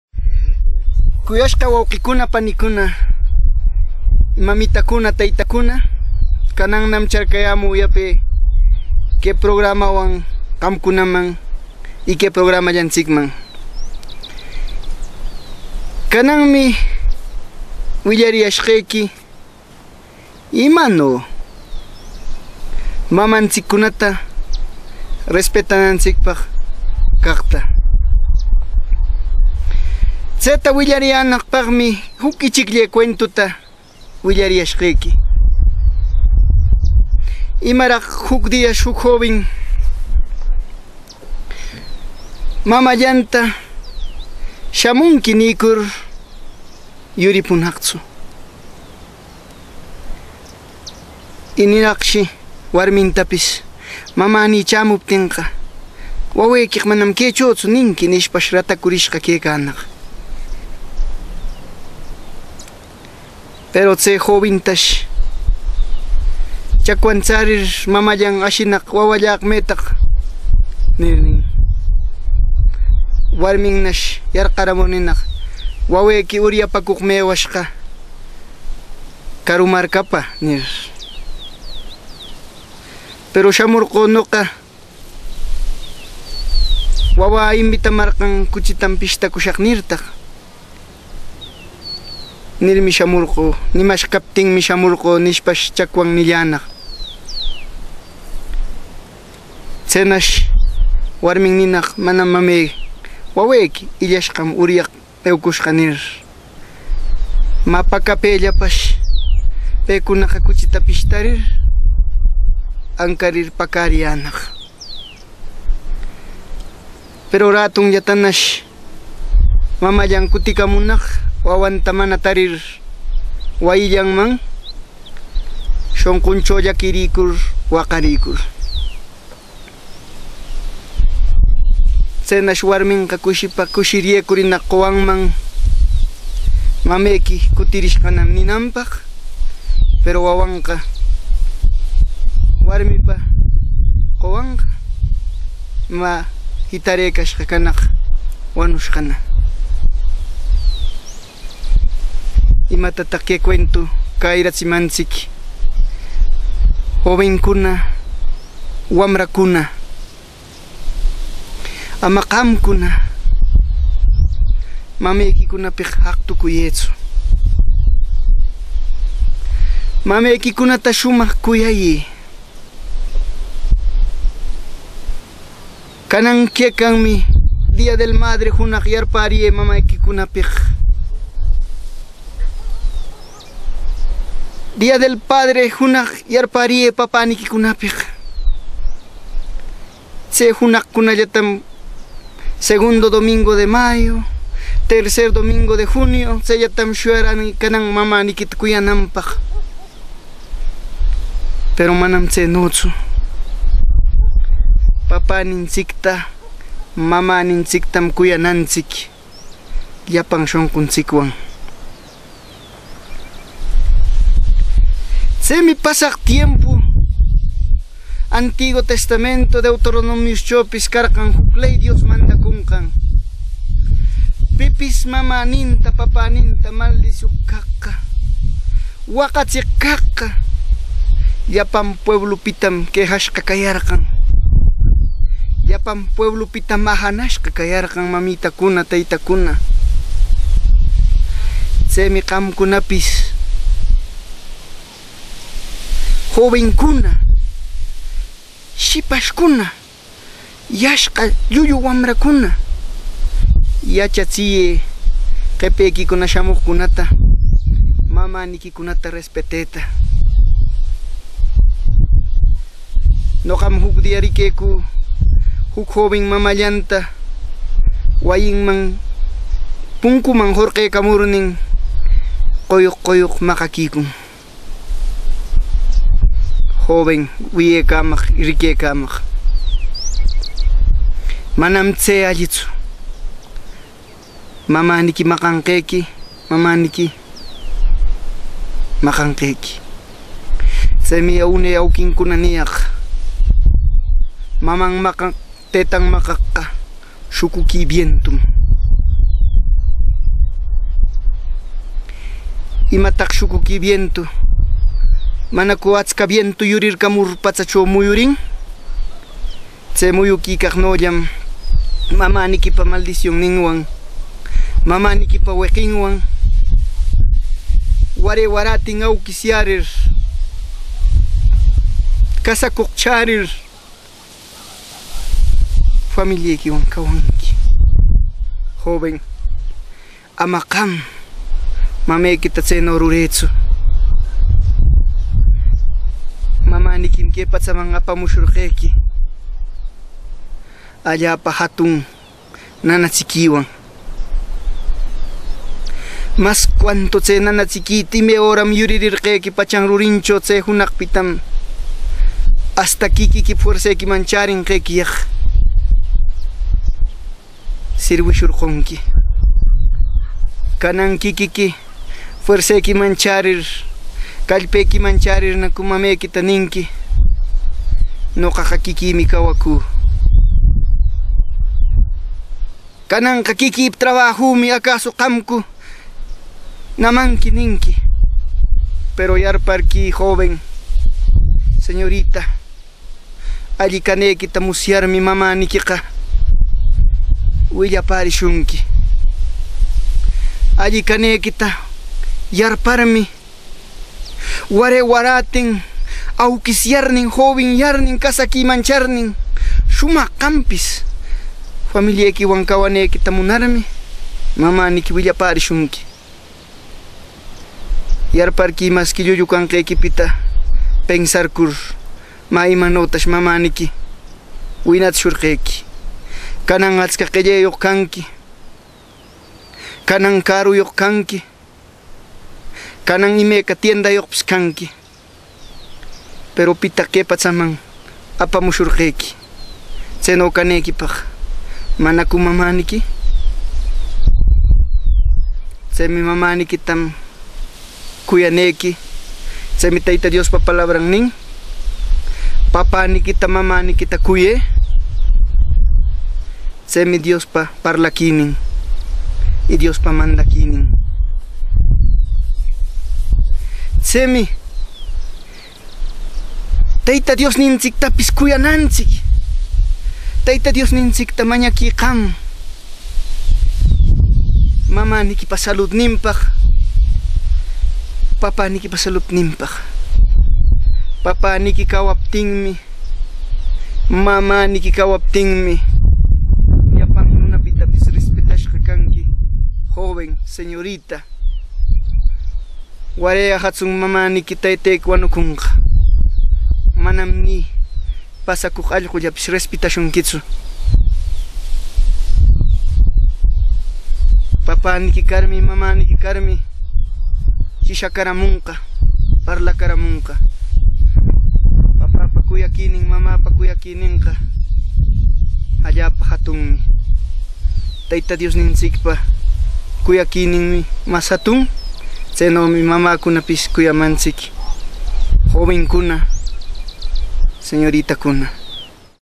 CUENTO JUC JOVIN MAMAMTA ULIPAM PRBLICADO POR ASOCIACIÓN AWI TELE-FAX 043 422650
MENSAJE EN QUECHUA DEL CONCHUCOS NORTE 2015 NARRADOR  VICENTE HUANUCO